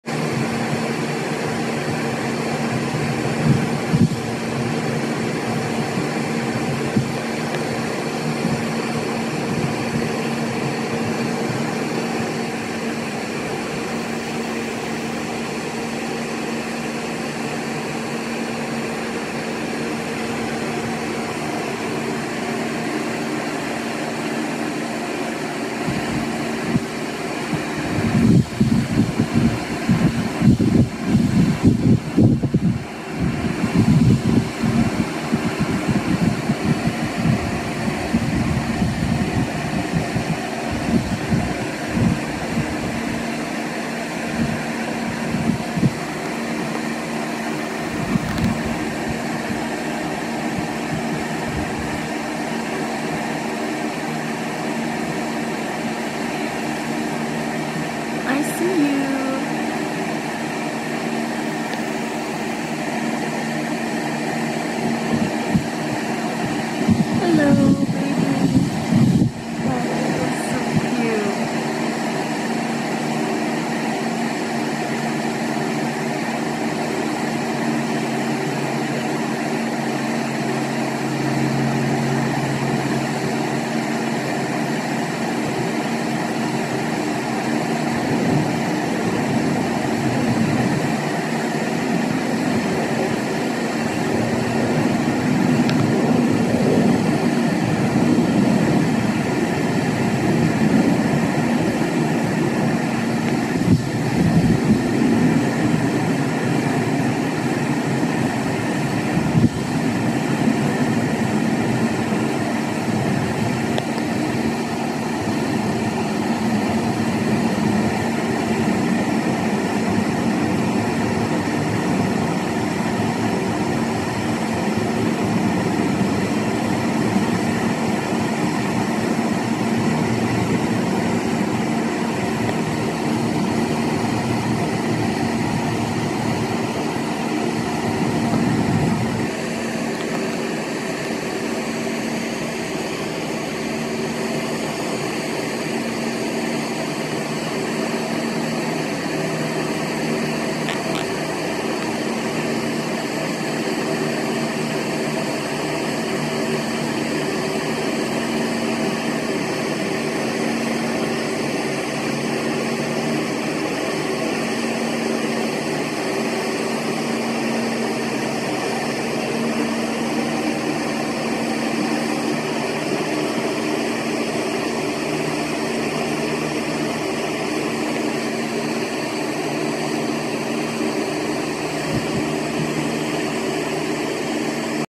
Raccoon 🦝 eating berries 🍒
raccoon 🦝 chilling eating some berries 🍒